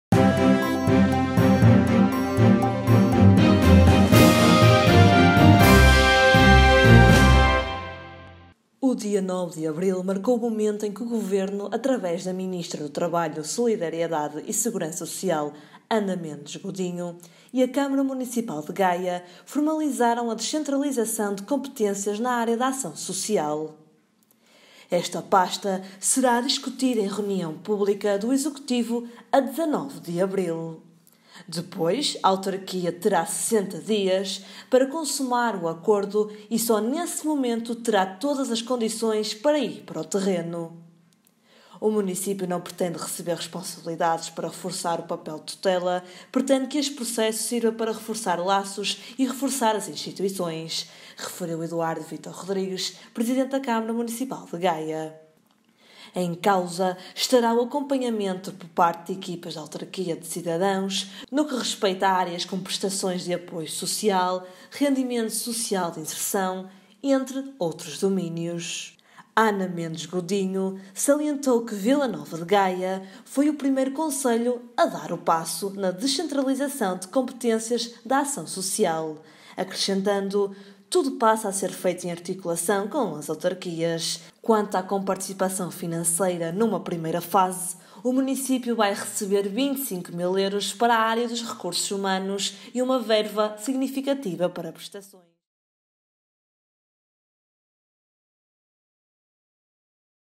Descentralização prestes a avançar na Ação Social
O dia 9 de abril marcou o momento em que o Governo, através da ministra do Trabalho, Solidariedade e Segurança Social, Ana Mendes Godinho, e a Câmara Municipal de Gaia formalizaram a descentralização de competências na área da Ação Social.